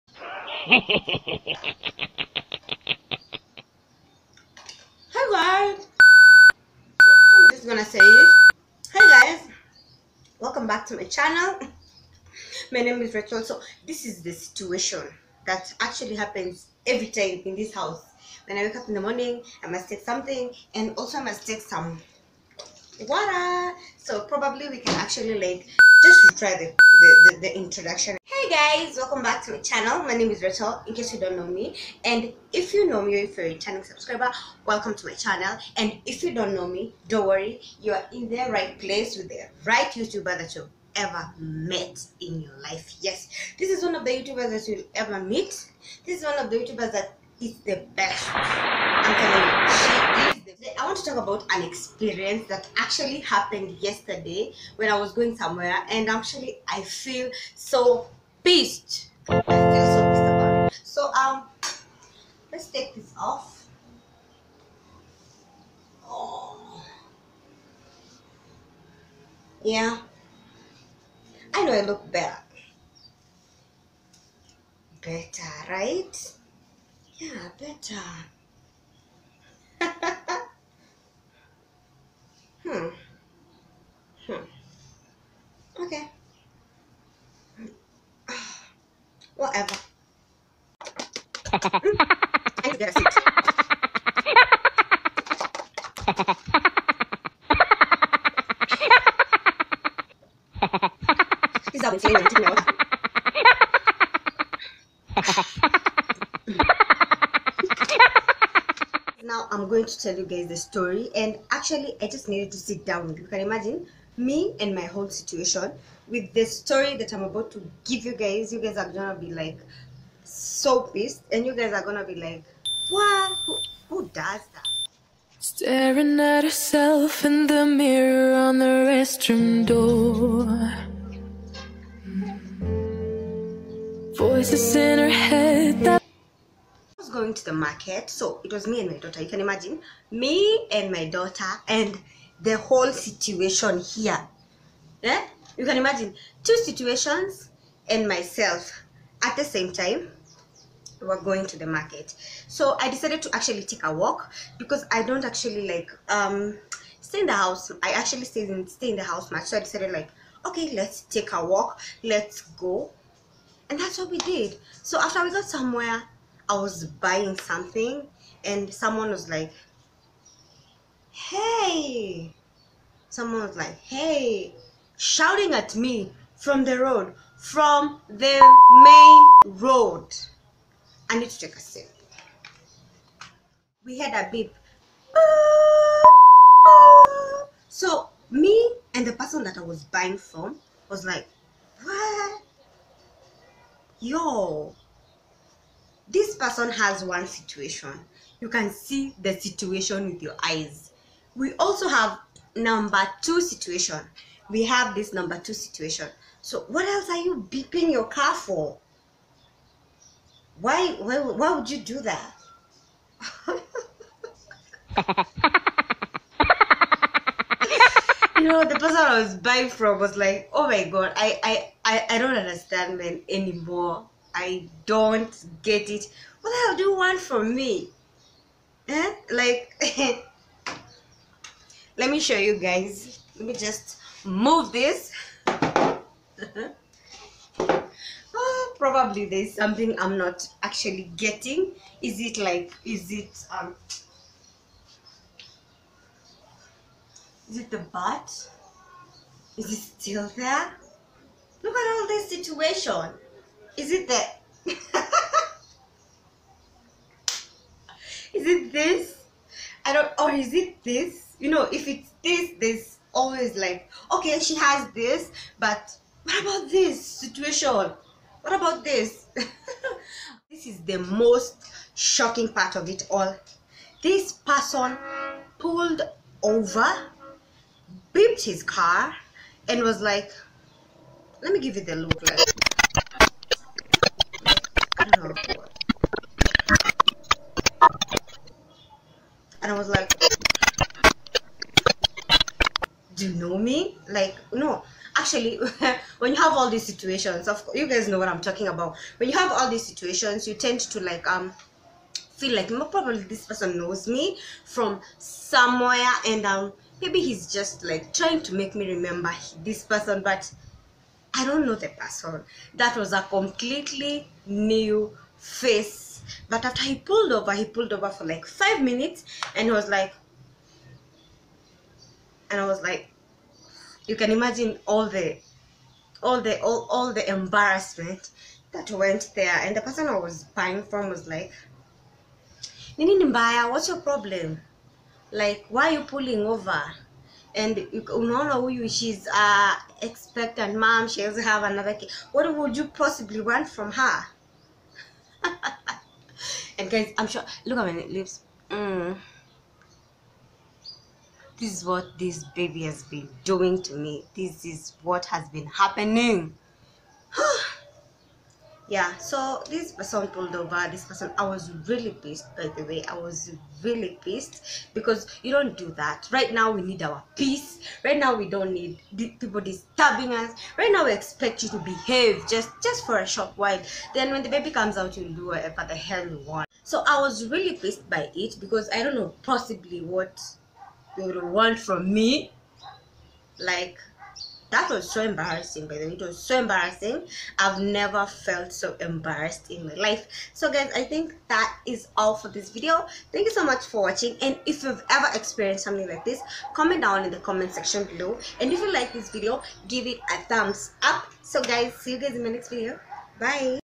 shocking!He pulled over for a pregnant woman?😡
HE PULLED OVER FOR A PREGNANT WOMAN

This video is basically a storytime video,about the most EMBARRASSING day of my life..hope you enjoy it.